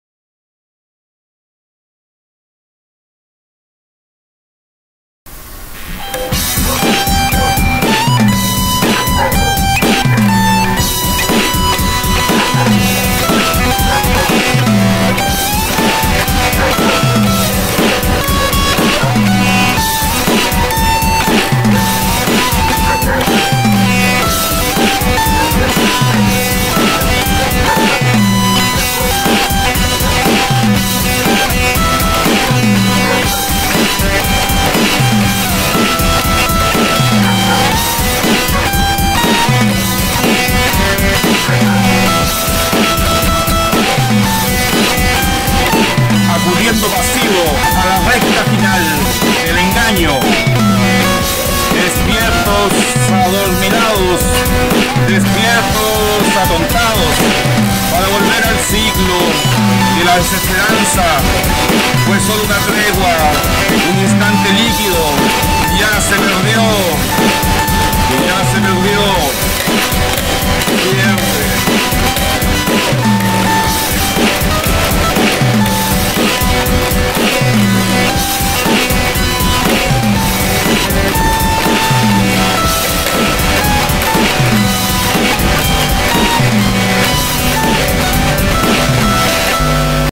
desengaño yurasiko
desengaño. empezando a aprender a manejar ruidos e interfaces, prueba